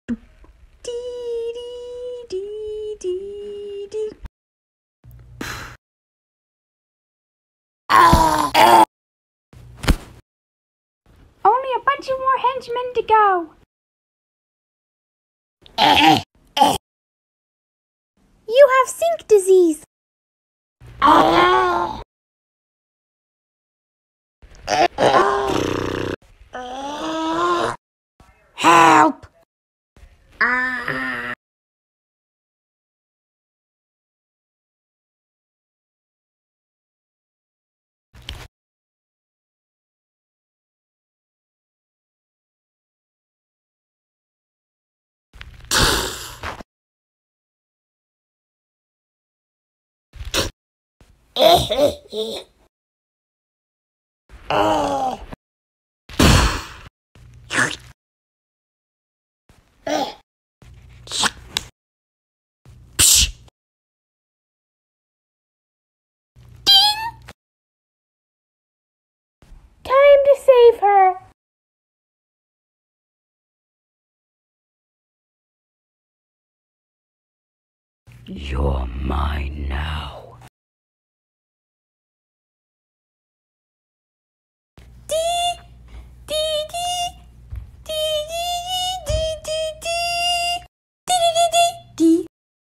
Pork Twins Animated Shorts: Corruption Part II
Porkette and Sonny fight all of the henchmen, but not all goes right...